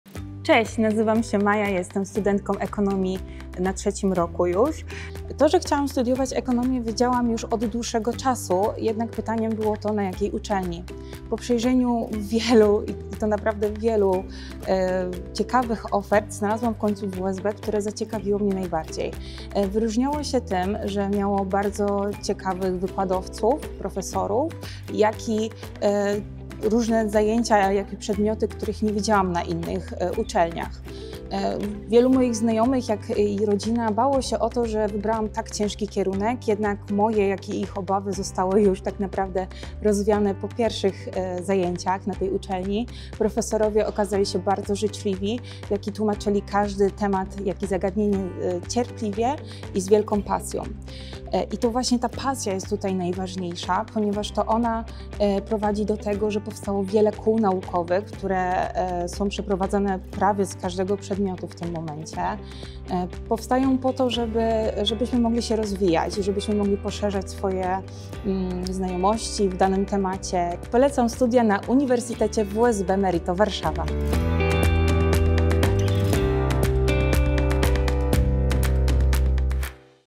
Miejsce, w którym pasja rodzi profesjonalizm – Uniwersytet WSB Merito Warszawa
Poznaj Uniwersytet WSB Merito Warszawa oczami studentki Mai. Według niej pasja to klucz do zrozumienia istoty naszej uczelni, a Ty co o tym uważasz? Podziel się swoją opinią w komentarzach!